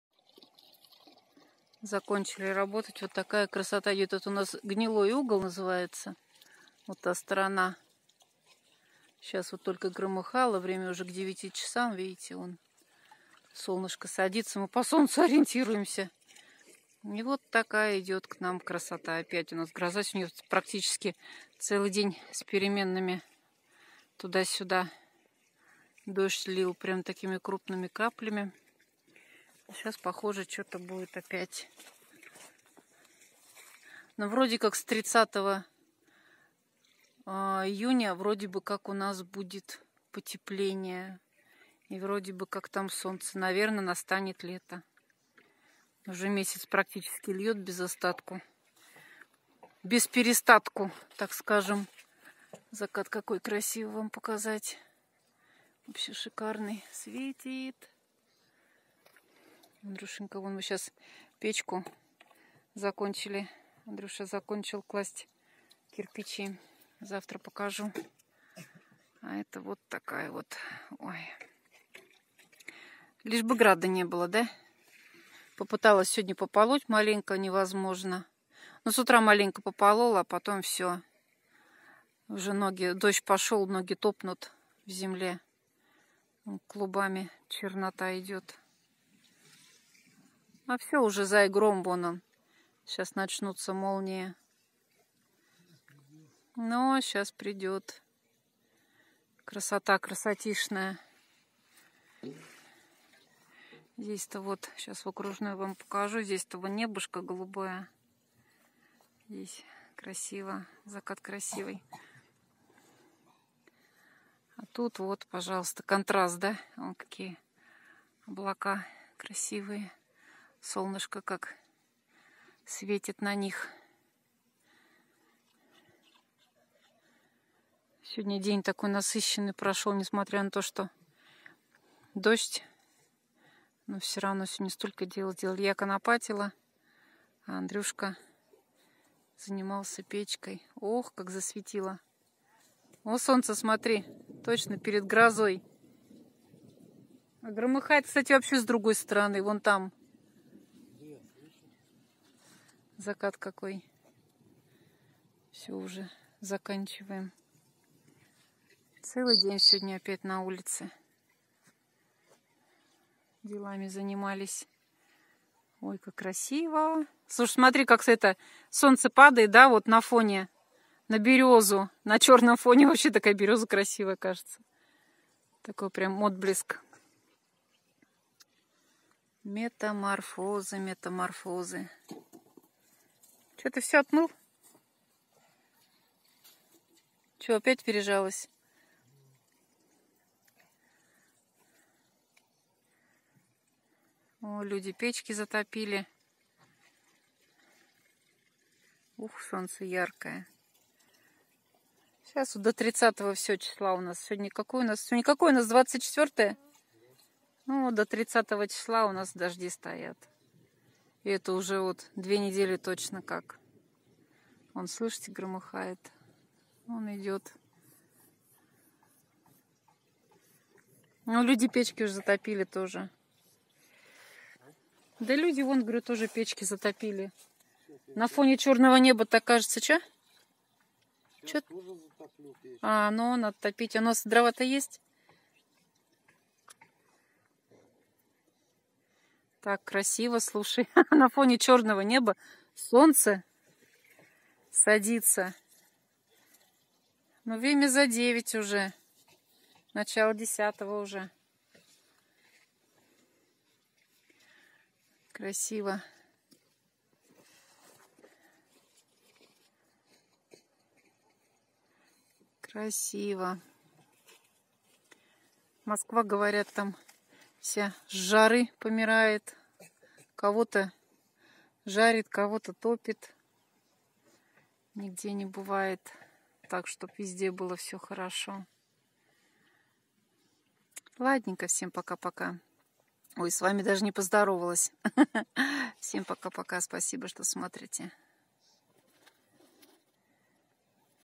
Красивый грозовой закат в деревне.Хороший день. закат,закатвдеревне,жизньвдеревне,грозовойзакат.
Очередной плодотворный день в деревне. Всегда наполнен делами. Скучать не приходится.закат,закатвдеревне,жизньвдеревне,грозовойзакатвдеревне.